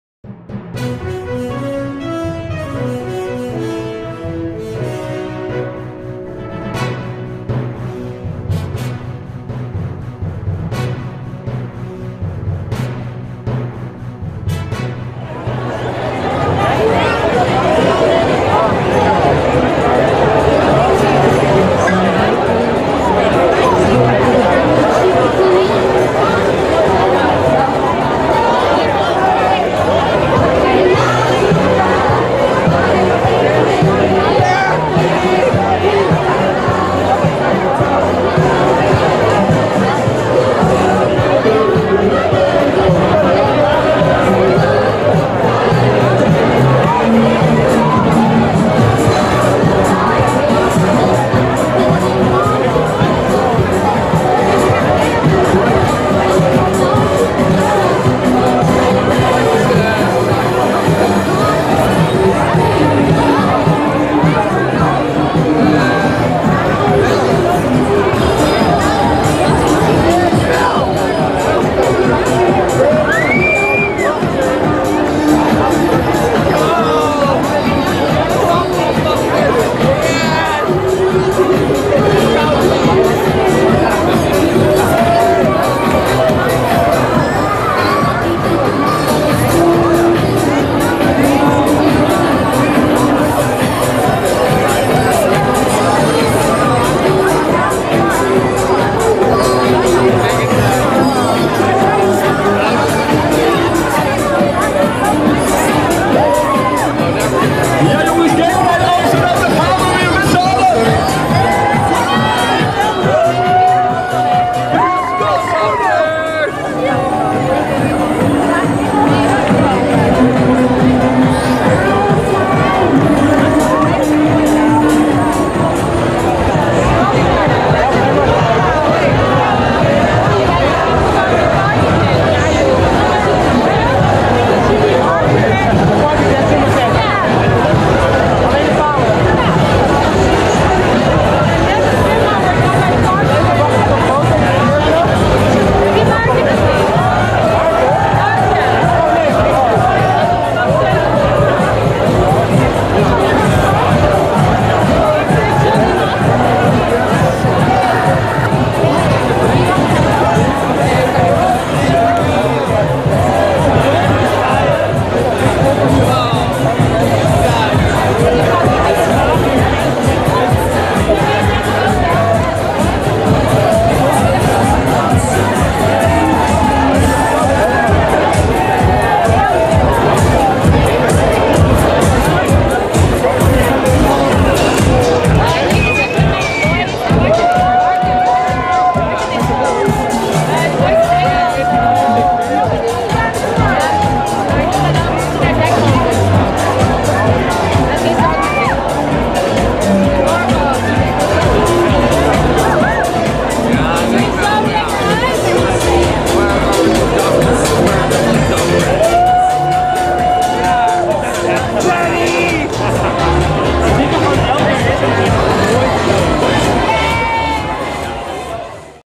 Amsterdam Pride 2022 Canal Parade / Botenparade: Staatsbosbeheer & Antoni van Leeuwenhoek
- De boten van Staatsbosbeheer by Rabobank en Antoni van Leeuwenhoek in de botenparade van Amsterdam Pride op zaterdagmiddag 6 augustus 2022.
- The Staatsbosbeheer by Rabobank and Antoni van Leeuwenhoek (cancer research) boats in the Amsterdam Pride Canal Parade, Saturday afternoon, August 6, 2022.